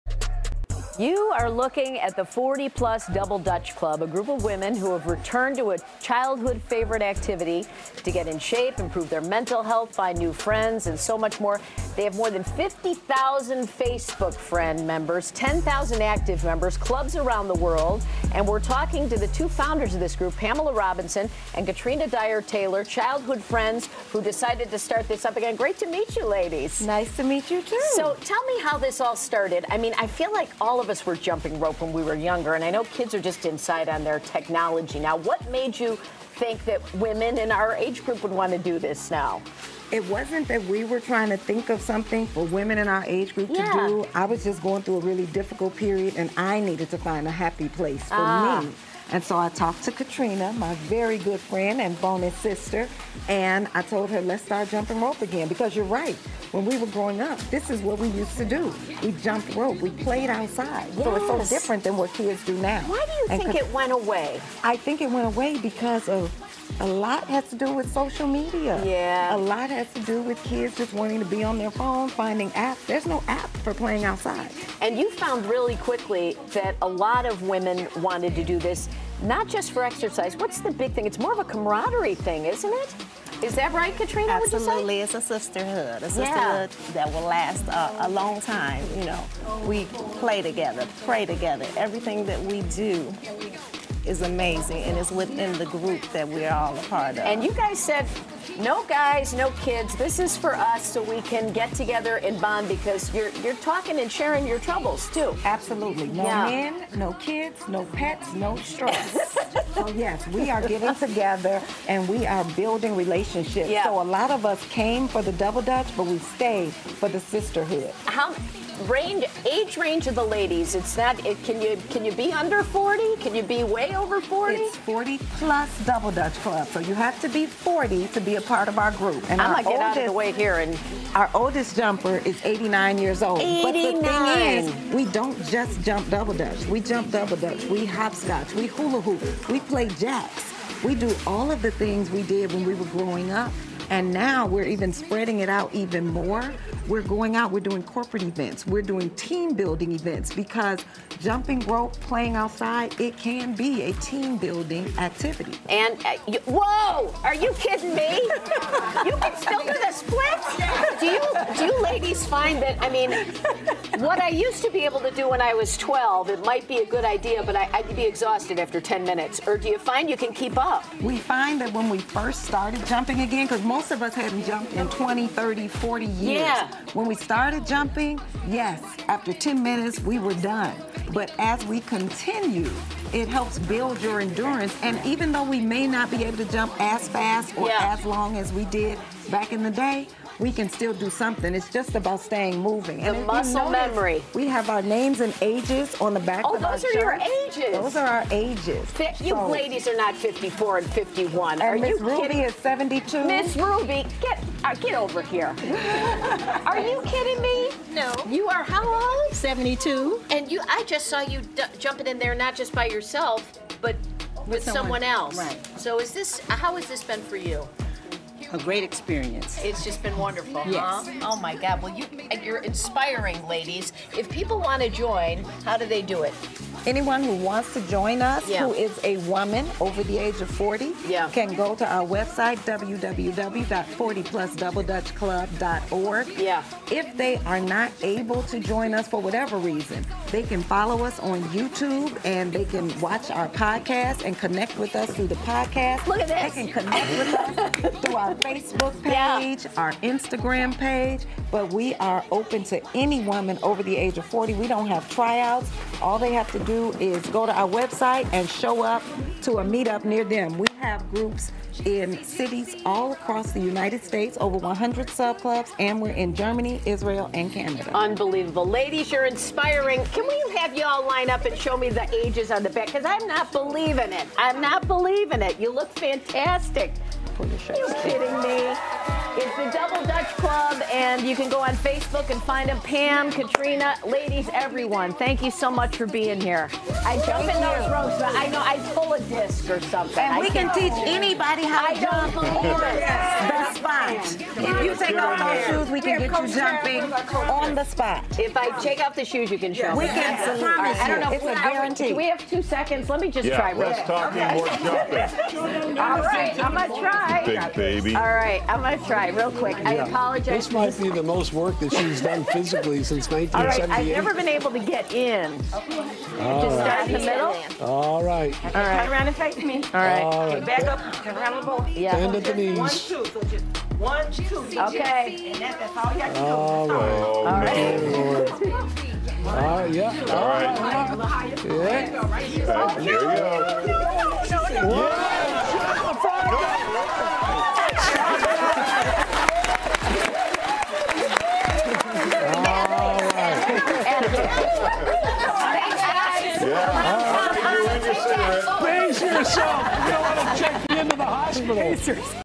40+ Double Dutch Club
A group of women age 40 and above who get together to relive old memories and create new ones while jumping double Dutch... their Facebook group page is over 50,000 and they have over 100 subClubs located in cities all over the United States including Chicago as well as in Germany, Canada and Israel